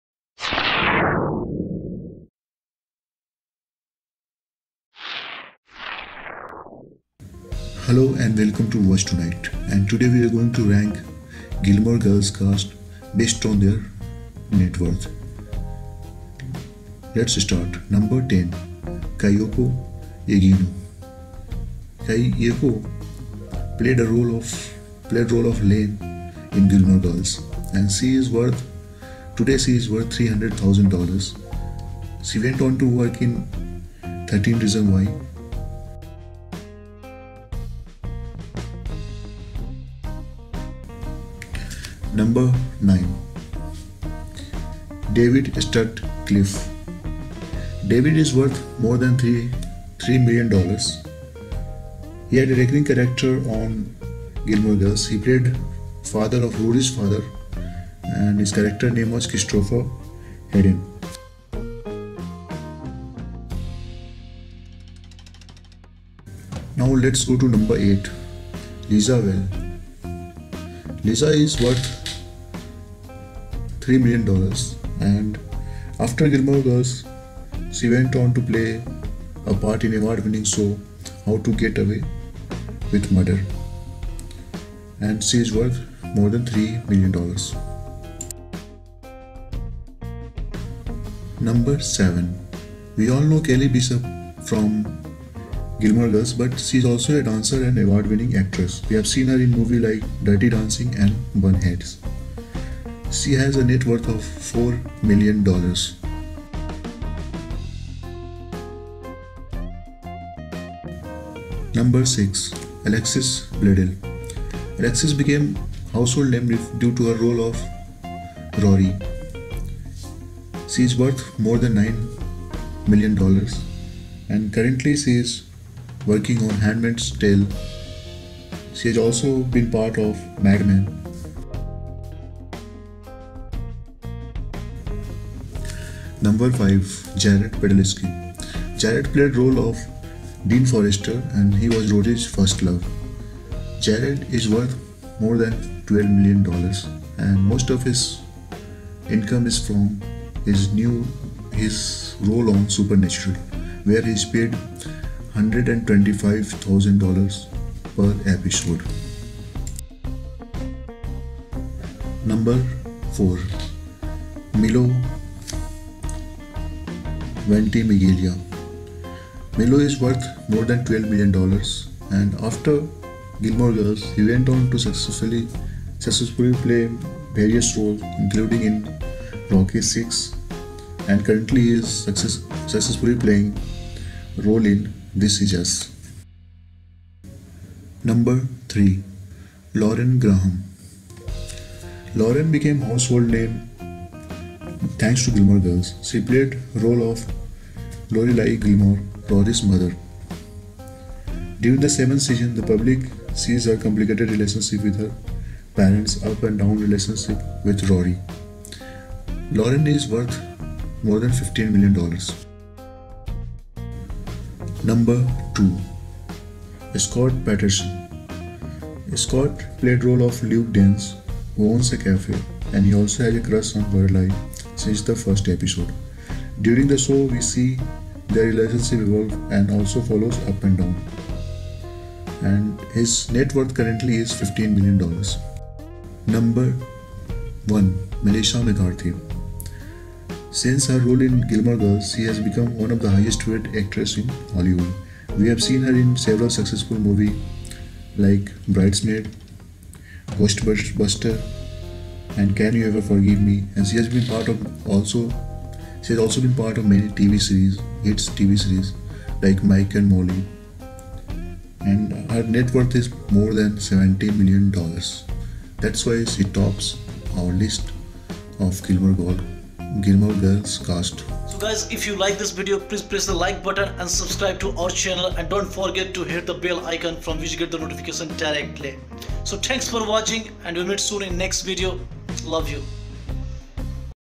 Gilmore girls Cast Networth | Ranked By Net Worth
Watch till end to find out net worth of Gilmore Girls Cast.
Alexis Bledel
Lauren Graham
Scott Patterson
Kelly Bishop
Melissa McCarthy
Keiko Agena
Jared Padalecki
Milo Ventimiglia
Sean Gunn
Liza Weil

Hope you having wonderful day.